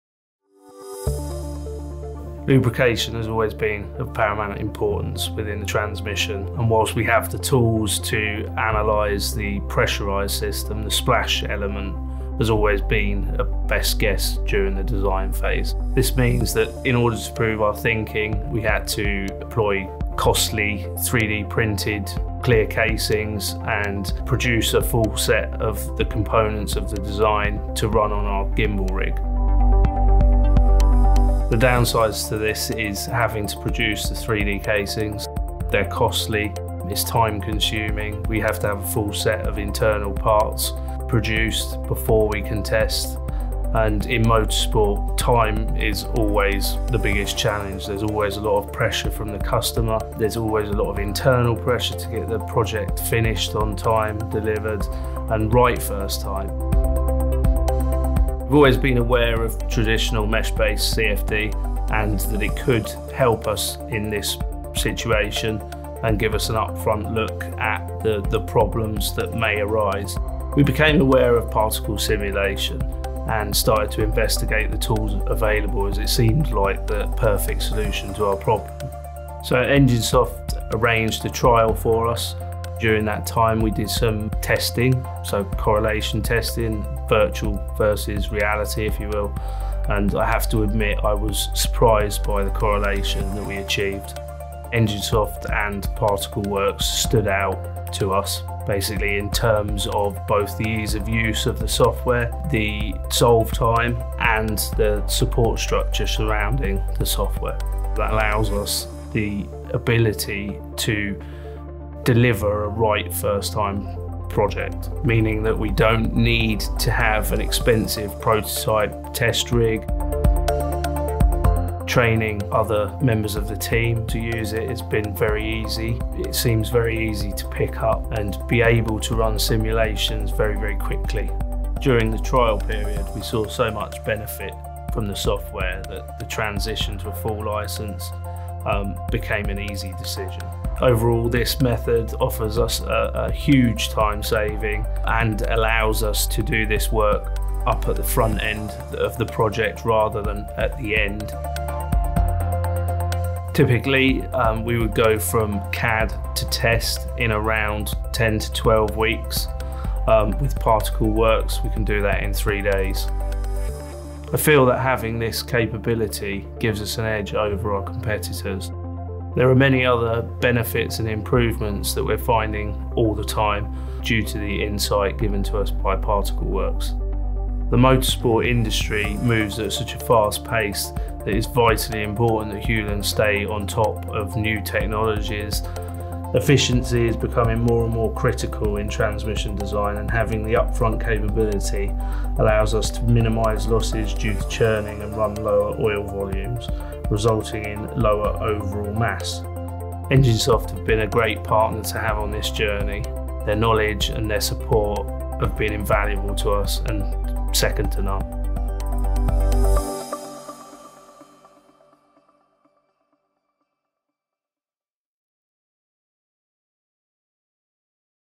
Particleworks Testimonial by Hewland
"EnginSoft have been a great partner to have on this journey, their knowledge have been invaluable to us and second to none"

In this video Hewland Engineering Ltd will provide an insight of their experience with EnginSoft UK & Particleworks, an advanced CFD Software exclusively distributed by us.

Discover the advantages and progressive prospects Particleworks can bring to your operations!

If you want further information on Particleworks, please get in touch with our expert consultants and engineers.